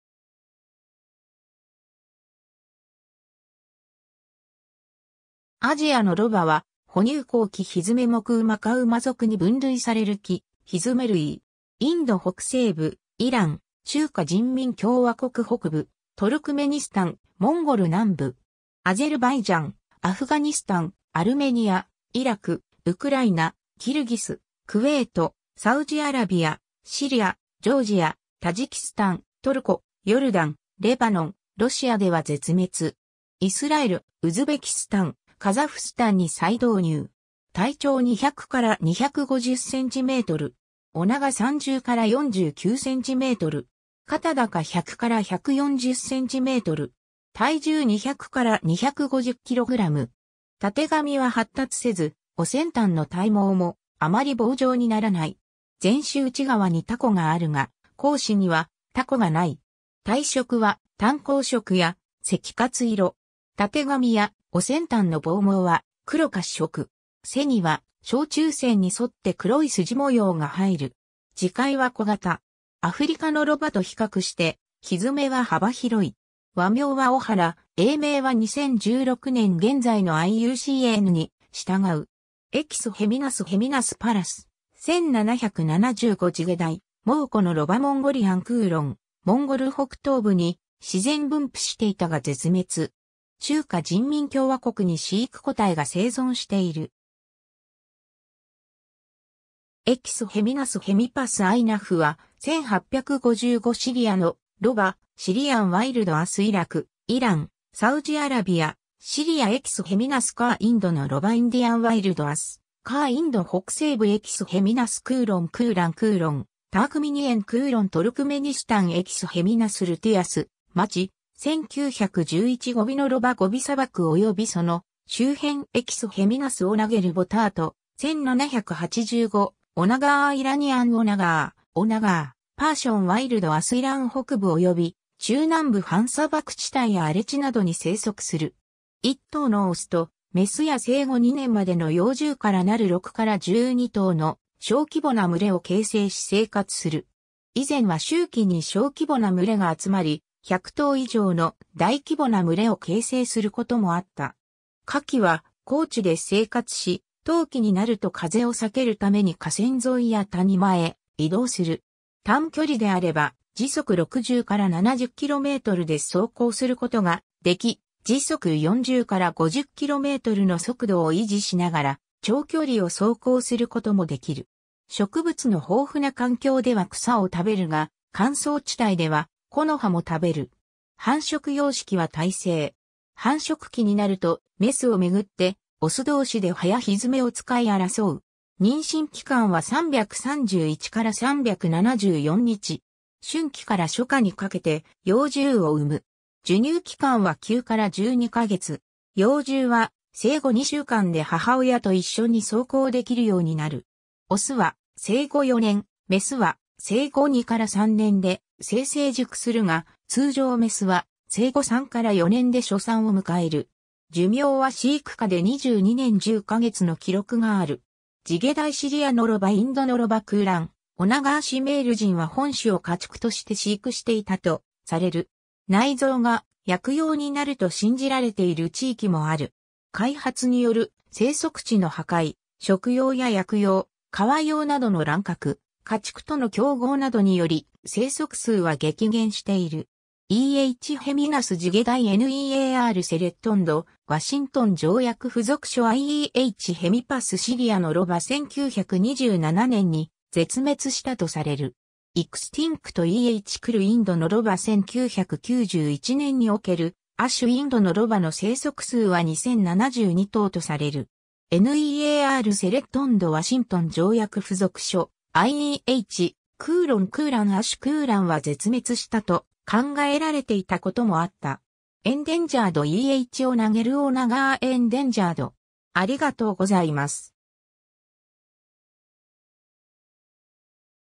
アジアノロバ
アジアノロバ（Equus hemionus）は、哺乳綱奇蹄目ウマ科ウマ属に分類される奇蹄類。
 インド北西部、イラン、中華人民共和国北部、トルクメニスタン、モンゴル南部。
アゼルバイジャン、アフガニスタン、アルメニア、イラク、ウクライナ、キルギス、クウェート、サウジアラビア、シリア、ジョージア、タジキスタン、トルコ、ヨルダン、レバノン、ロシアでは絶滅。
イスラエル、ウズベキスタン、カザフスタンに再導入。
 体長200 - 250センチメートル。
尾長30 - 49センチメートル（房毛除く）。
肩高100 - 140センチメートル。
鬣は発達せず、尾先端の体毛もあまり房状にならない。
前肢内側にたこがあるが、後肢にはたこがない。
体色は淡黄色や赤褐色。
鬣や尾先端の房毛は黒褐色。
背には正中腺に沿って黒い筋模様が入る。
 耳介は小型。
アフリカノロバと比較して蹄は幅広い。
 和名は小原(2000)、英名は2016年現在のIUCNに従う。
 Equus hemionus hemionus Pallas, 1775 ジゲダイ、モウコノロバ Mongolian Kulan モンゴル北東部に自然分布していたが絶滅。
中華人民共和国に飼育個体が生存している。
 †Equus hemionus hemippus I. Geoffroy, 1855 シリアノロバ Syrian wild ass（絶滅亜種） イラク、イラン、サウジアラビア、シリア Equus hemionus khur (Lesson, 1827) インドノロバ Indian wild ass, Khur インド北西部 Equus hemionus kulan (Groves & Mazak, 1967) クーラン Kulan, Turkmenian kulan トルクメニスタン Equus hemionus luteus Matschie, 1911 ゴビノロバ ゴビ砂漠およびその周辺 Equus hemionus onager Boddaert, 1785 オナガー Iranian Onager, Onager, Percian wild ass イラン北部および中南部 半砂漠地帯や荒れ地などに生息する。
1頭のオスと、メスや生後2年までの幼獣からなる6 - 12頭の小規模な群れを形成し生活する。
以前は秋季に小規模な群れが集まり、100頭以上の大規模な群れを形成することもあった。
夏季は高地で生活し、冬季になると風を避けるために河川沿いや谷間へ移動する。
短距離であれば時速60 - 70キロメートルで走行することができ、時速40 - 50キロメートルの速度を維持しながら長距離を走行することもできる。
 植物の豊富な環境では草を食べるが、乾燥地帯では木の葉も食べる。
 繁殖様式は胎生。
繁殖期になるとメスを巡ってオス同士で歯や蹄を使い争う。
妊娠期間は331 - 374日。
春季から初夏にかけて幼獣を産む。
授乳期間は9 - 12か月。
幼獣は生後2週間で母親と一緒に走行できるようになる。
オスは生後4年、メスは生後2-3年で性成熟するが、通常メスは生後3-4年で初産を迎える。
寿命は飼育下で22年10か月の記録がある。
 ジゲダイ シリアノロバ インドノロバ クーラン オナガー シュメール人は本種を家畜として飼育していたとされる。
内臓が薬用になると信じられている地域もある。
 開発による生息地の破壊、食用や薬用・皮用などの乱獲、家畜との競合などにより生息数は激減している。
 E. h. hemionus ジゲダイ NEAR THREATENED (IUCN Red List Ver. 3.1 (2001)) ワシントン条約附属書I E. h. hemippus シリアノロバ 1927年に絶滅したとされる。
 EXTINCT (IUCN Red List Ver. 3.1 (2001)) E. h. khul インドノロバ 1991年における亜種インドノロバの生息数は2,072頭とされる。
 NEAR THREATENED (IUCN Red List Ver. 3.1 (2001)) ワシントン条約附属書I E. h. kulan クーラン 亜種クーランは絶滅したと考えられていたこともあった。
 ENDAN...